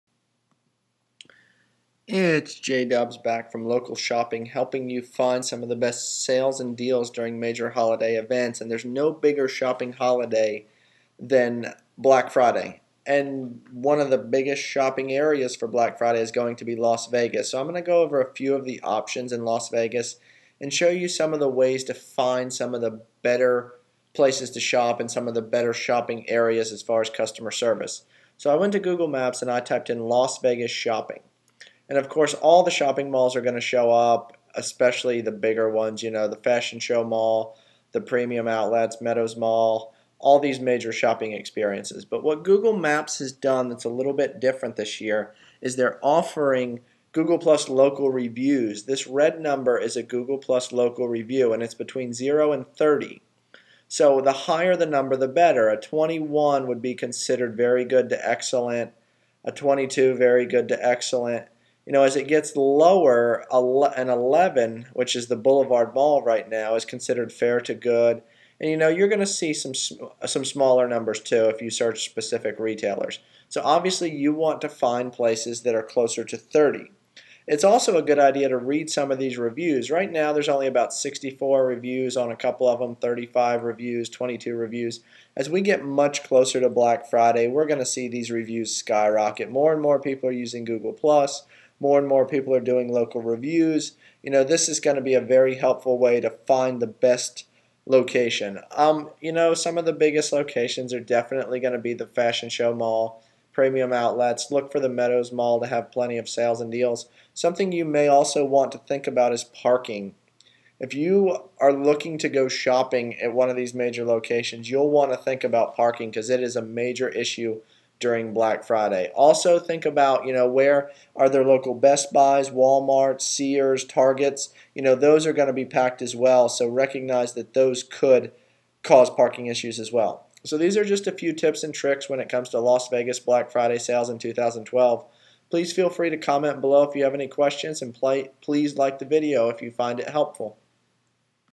Las Vegas Black Friday Sales 2012 - Fashion Show Mall, Premium Outlets and Meadows Mall
- With Black Friday 2012 just around the corner millions of hard working Americans will look for the best sales and deals.  Some of the most popular malls will include the Fashion Show Mall, Premium Outlets and Meadows Mall.  Remember parking will be an issue so think ahead.